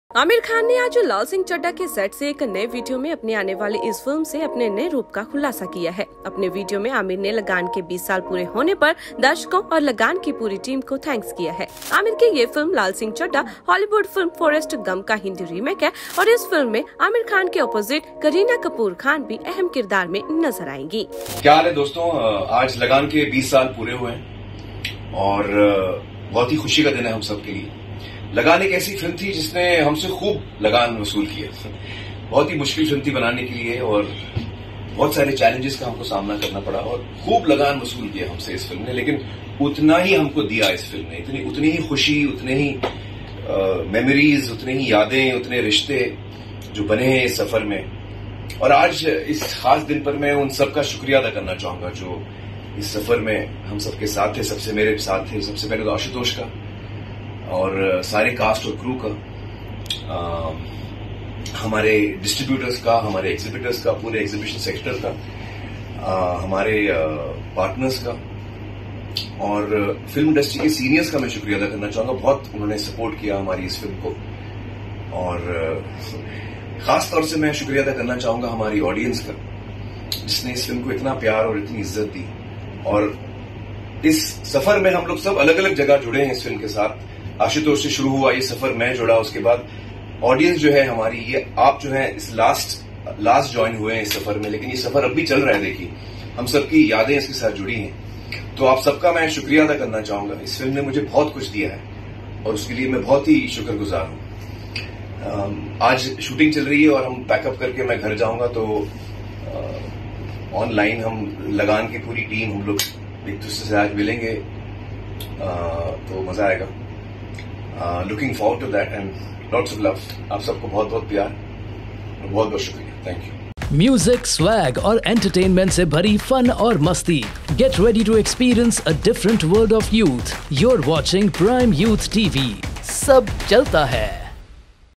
AAMIR KHAN कैमरा के सामने हुए EMOTIONAL | Chalo Chale Lagaan | Bollywood Latest News |Prime Youth TV
It has been 20 years since Aamir Khan‘s Oscar-nominated period drama Lagaan hit theatres. And on this special occasion, the Bollywood star reminisced about the experience of making the film in a video posted on Aamir Khan Productions’ Instagram handle.

Aamir Khan shared a video from the sets of lal singh chaddha movie, In the video, Aamir, dressed in military uniform, thanked filmmaker Ashutosh Gowariker for giving him the chance to be a part of such an epic story.

“Firstly, I want to thank Ashutosh (Gowariker), and the entire cast and crew, and the partners as well as the entire exhibitor sector for their support. I also want to extend my thanks to the seniors of the film industry who stood behind the project and showered so much praise on it,” the actor said.

At one point, Aamir Khan also made light of the challenges posed by Lagaan. He said, “Lagaan was a film that made sure we paid our Lagaan in full. It was a very difficult film to make. We had to face a lot of hardships, but the film also gave the team a lot of unforgettable memories, relationships and happiness.”